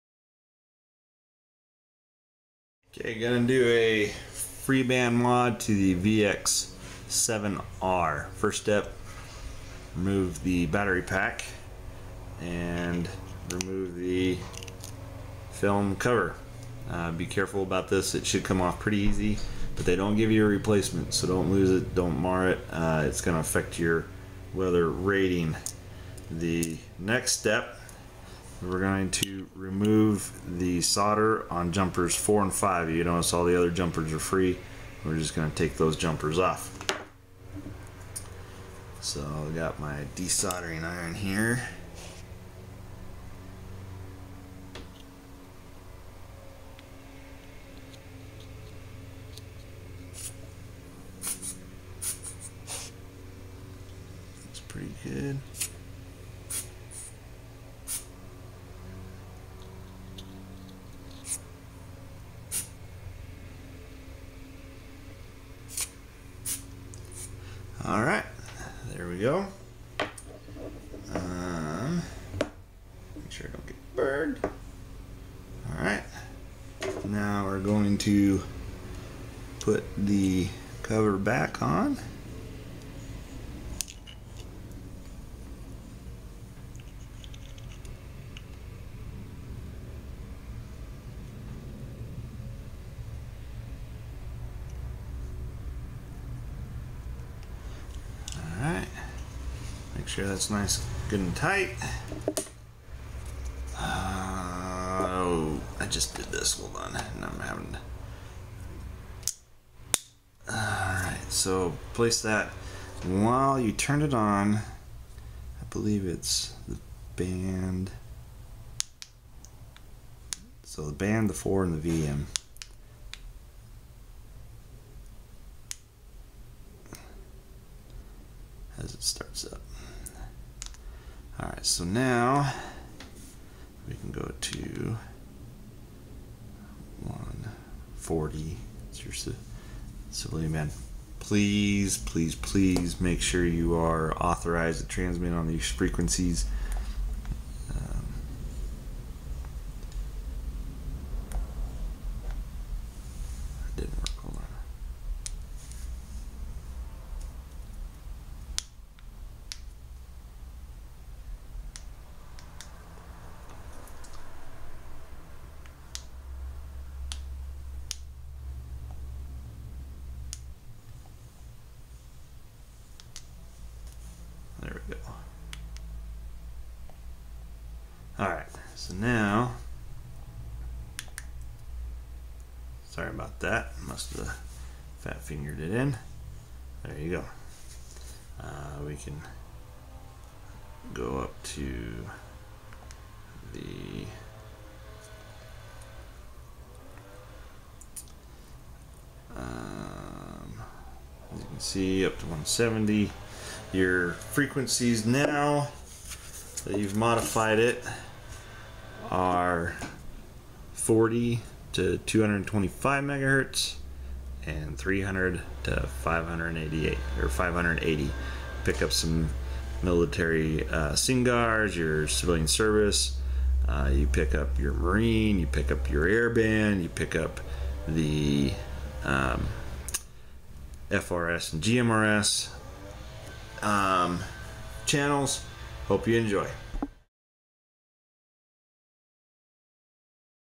Yaesu VX-7R Out of Band Modification
How to do an out of band modification to expand the functionality of the Yaesu VX-7R.  Pretty simple, just make sure that you only transmit on frequencies you are legally authorized to transmit on!
New transmit frequency is 40-225 MHz and 300-580 MHz.  Receive frequencies does not change.